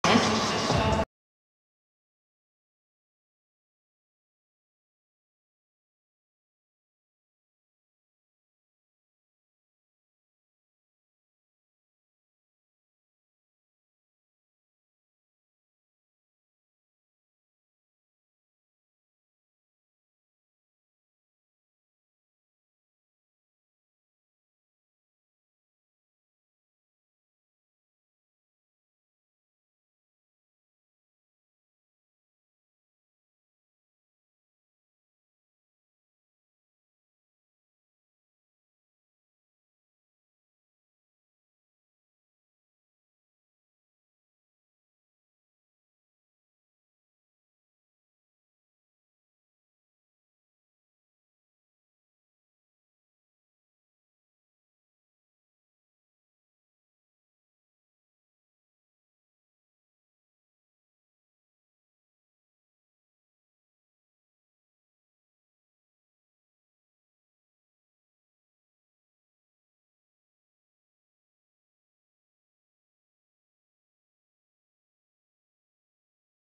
【ライオンズ】長谷川 信哉（敦賀気比・2年目）のフリーバッティング
2022年3月2日 読売ジャイアンツ 対 埼玉西武ライオンズ